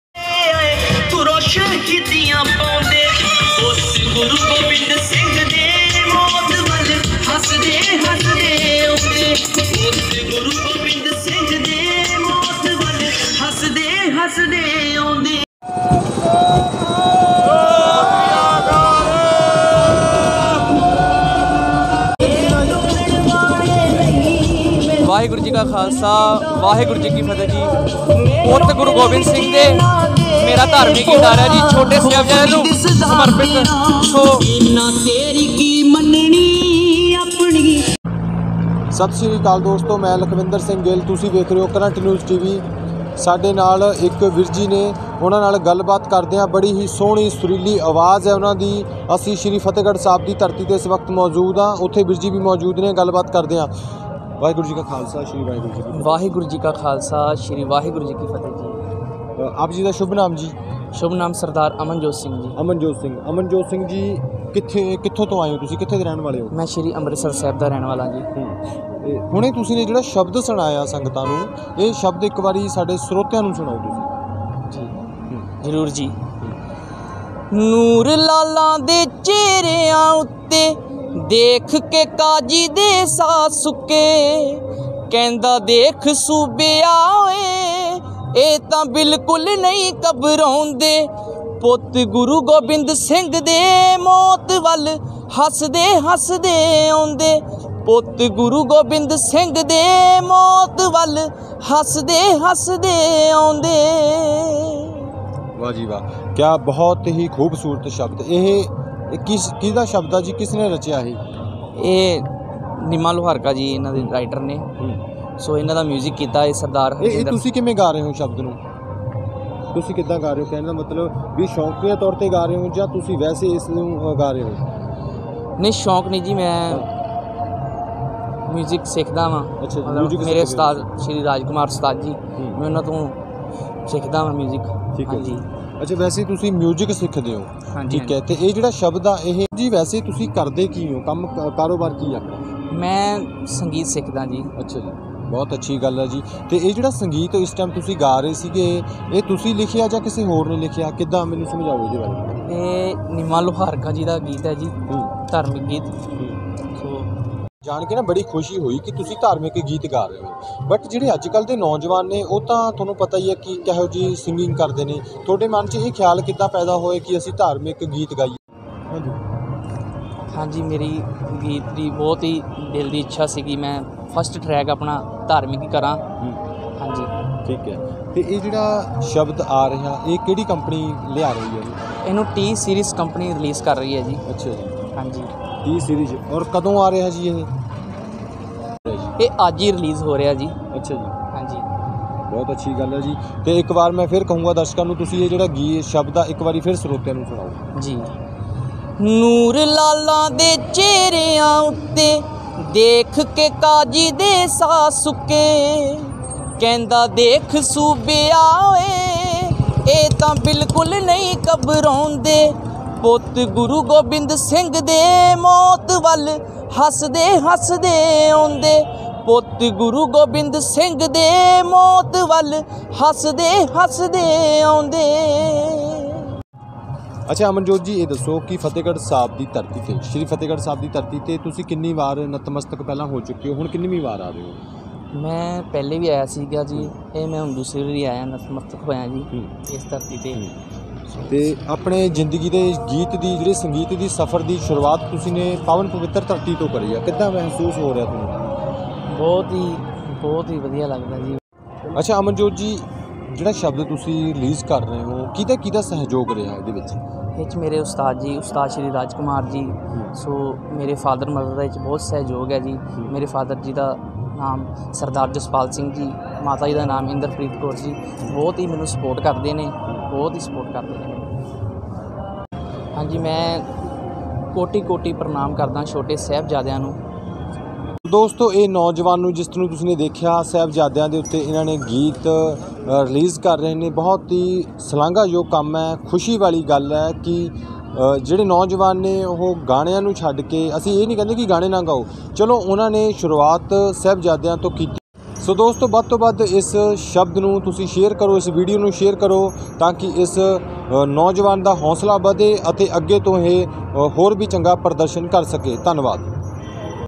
Putt Guru Gobind Singh de Singer Amanjot Singh interview with Lakhwinder Singh gill
Current News Tv with Lakhwinder Singh gill